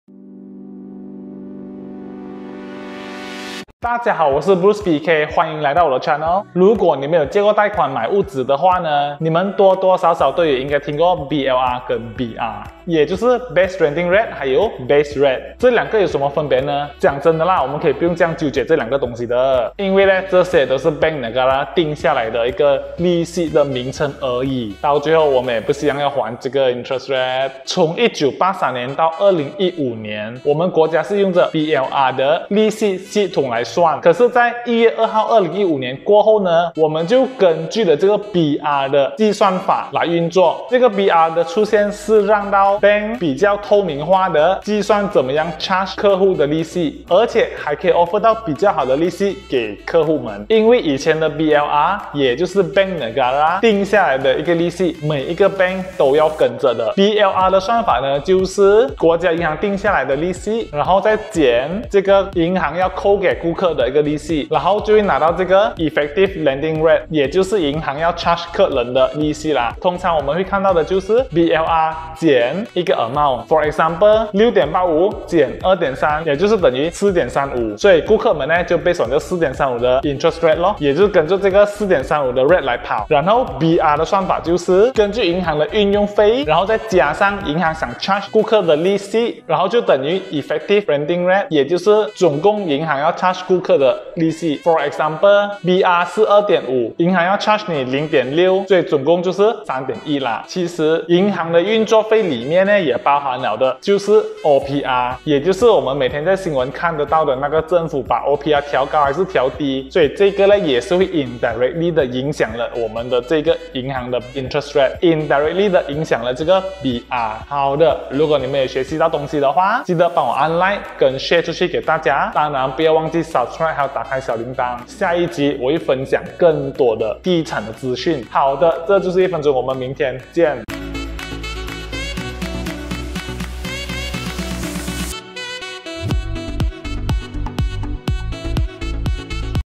Day 32 【1日1分钟】 你们知道的 BLR 和 BR【利息】的分别和 【OPR】
Day 32 【1日1分钟】 你们知道 【BLR v BR】
银行的 【利息】的分别和 【OPR】
【BLR v BR】的关系

【东马朋友】，或者是其他州的朋友
想投资 KL 【必】看